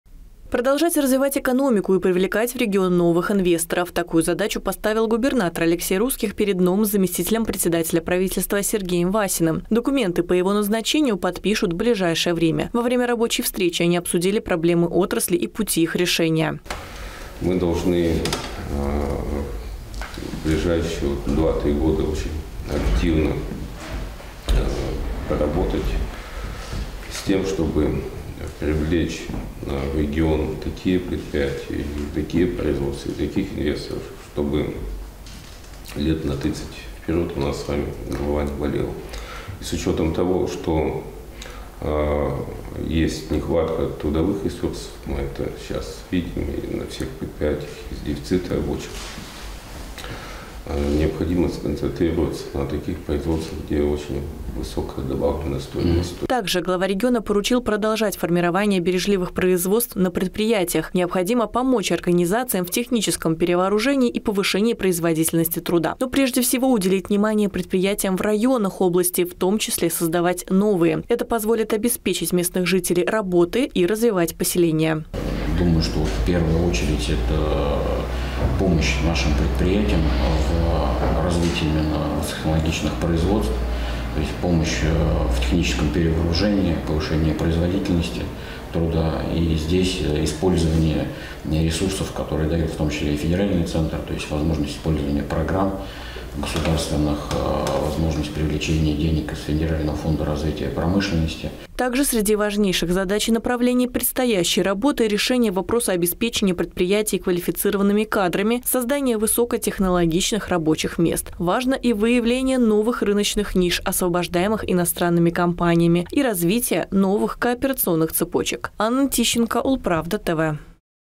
Новые производства и рабочие места. Что обсудили Алексей Русских с Сергеем Васиным
На этой неделе глава региона Алексей Русских озвучил ряд кадровых изменений в правительстве. В том числе отрасль промышленности доверили курировать Сергей Васин, много лет возглавлявший региональную корпорацию развития. Теперь он продолжит работу в этом направлении в качестве заместителя председателя правительства. План работы обсудили на встрече с губернатором.